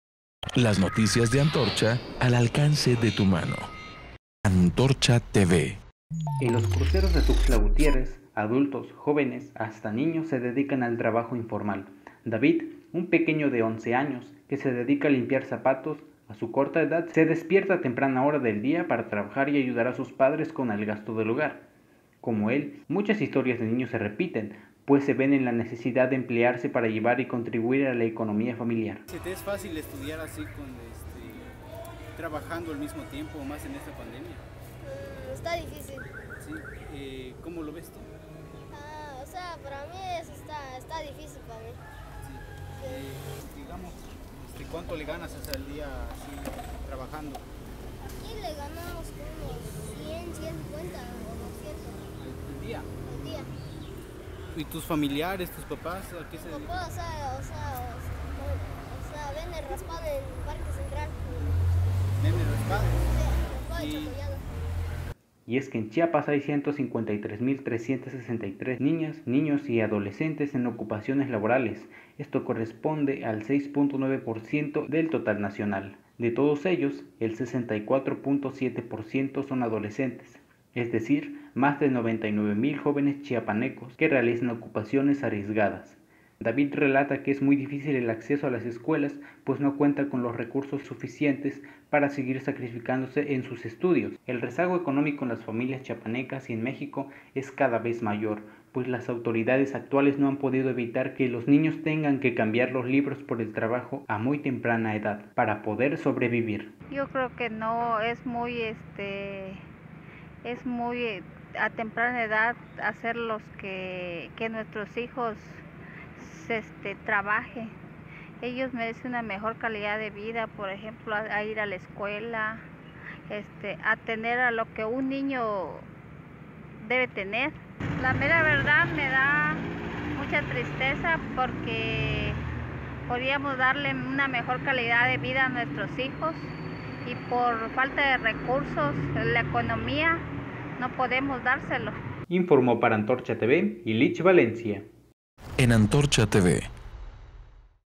Crece trabajo infantil en Chiapas, autoridades no garantizan desarrollo de menores
El rezago económico en las familias chiapanecas es cada vez mayor y dificulta que los niños puedan tener acceso a derechos convertidos en privilegios, como la educación.